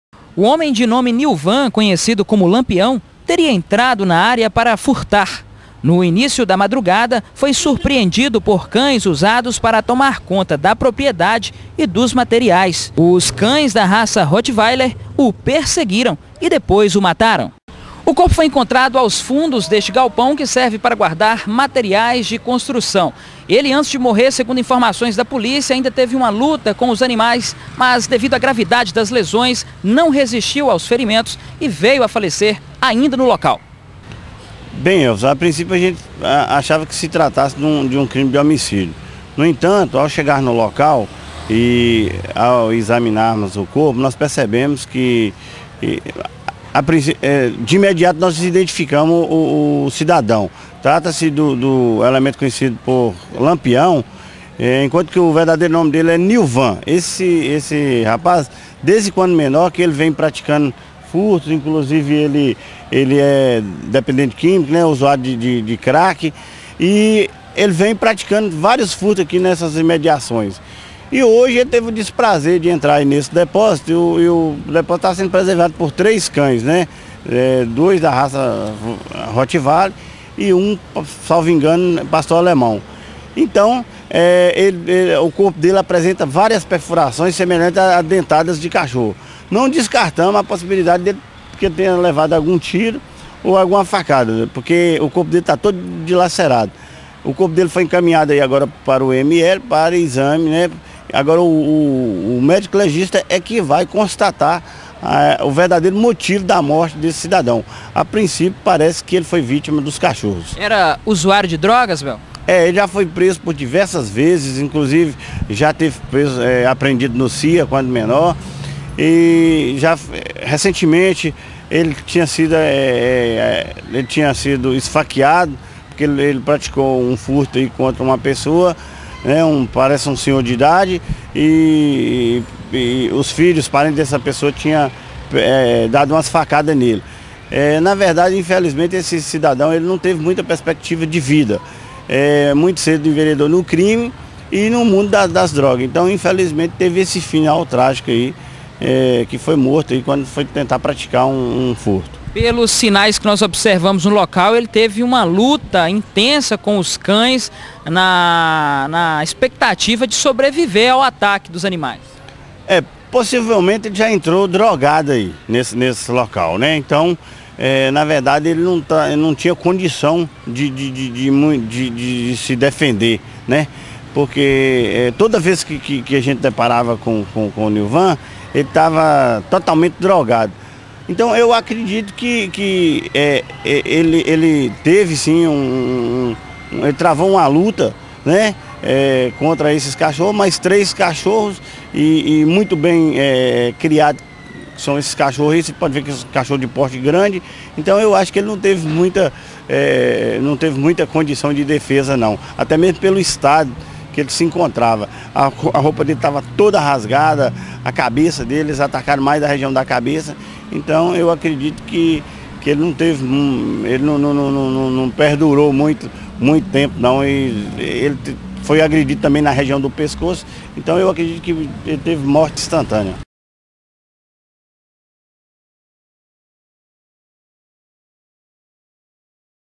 Tv Imigrantes ( Cães matam ladrão em Teofilo Otoni)
Três cachorros mataram um ladrão que tentava furtar materiais de construção , reportagem de Elvis Passos e Kassem Said